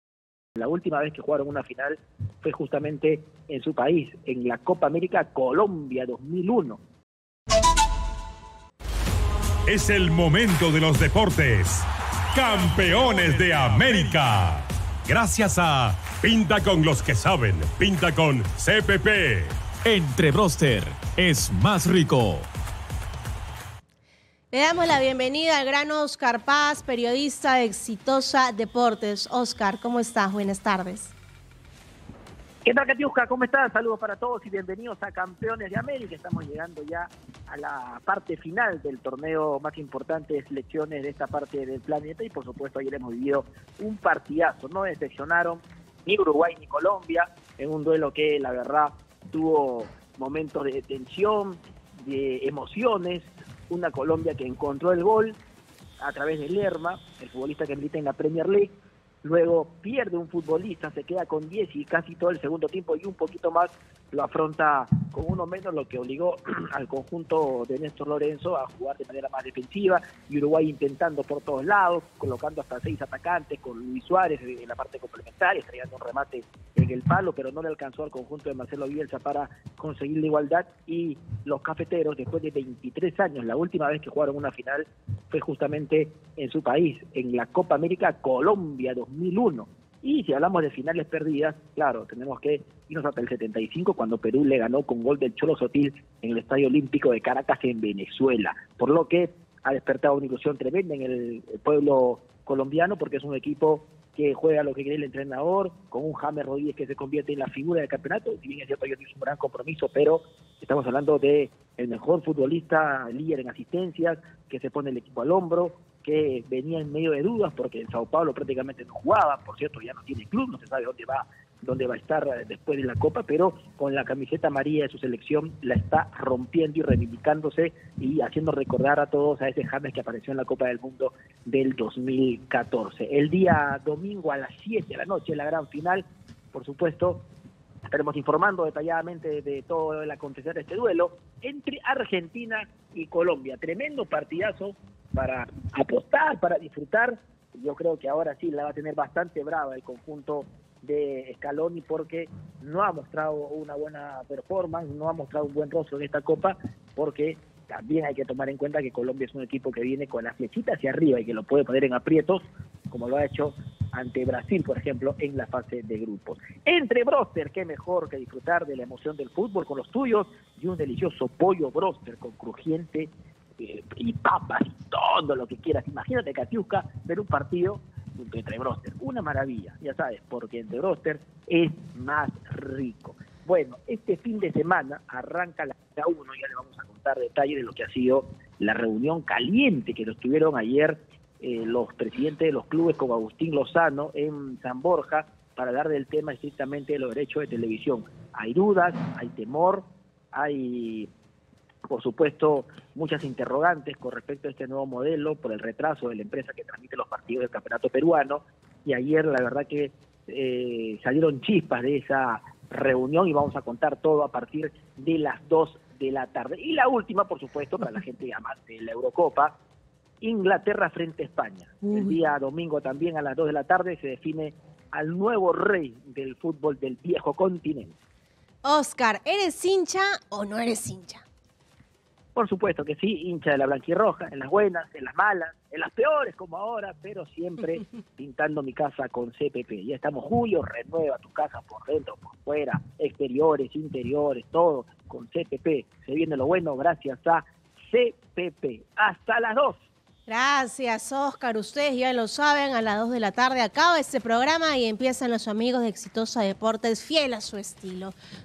🔴🔵Final de Copa América 2024: Argentina vs. Colombia este domingo 7:00 p.m.
El periodista de Exitosa Deportes, Óscar Paz, se pronunció sobre el partido entre Argentina y Colombia por la final de la Copa América este domingo a las 7:00 de la noche.

Exitosa Perú
"Exitosa: La voz que integra al Perú"

Corporación Universal